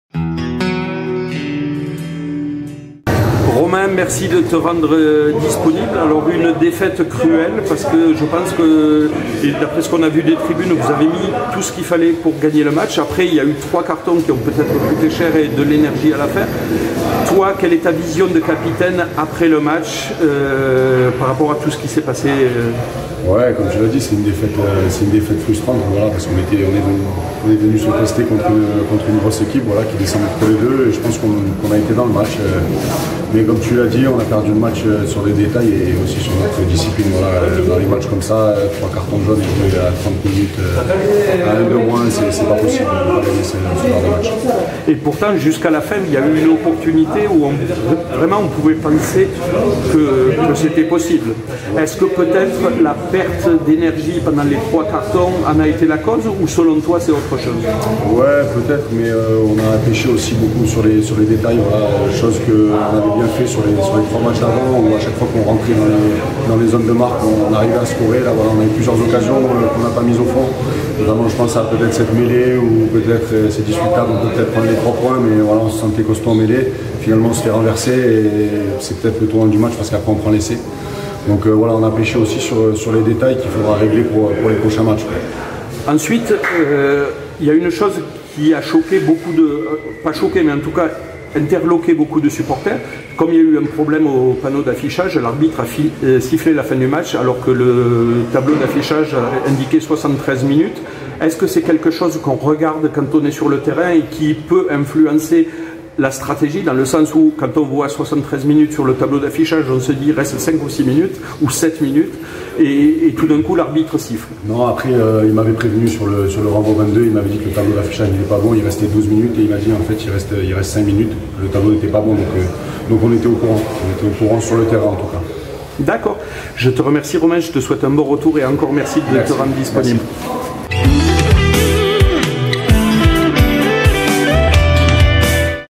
Interview d'après match - J02 - Saison 2024 2025 - RNR USC - Romain MANCHIA - 20240831
RNR - USC
Interview d'après match le 31/08/2024 au Stade Robert Diochon de Rouen. Romain Manchia, deuxième ligne et capitaine de l'USC répond aux questions des Hérétiques après la courte défaite de nos valeureux guerriers en terre normande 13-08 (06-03 à la mi-temps) dans le cadre de la 2ème journée du championnat de Nationale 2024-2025.